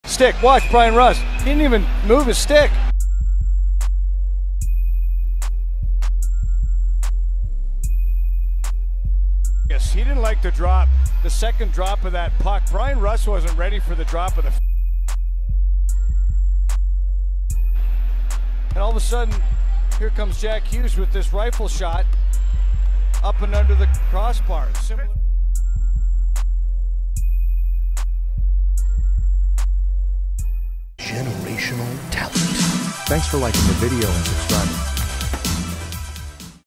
Mike Sullivan is ANGRY Like Torts
Got a bench minor for this. Mad at referee. Bryan Rust forgets how to play hockey too. Pens lose 4-2 to New Jersey Devils.
Official GLOVEY T-Shirt NOW AVAILABLE!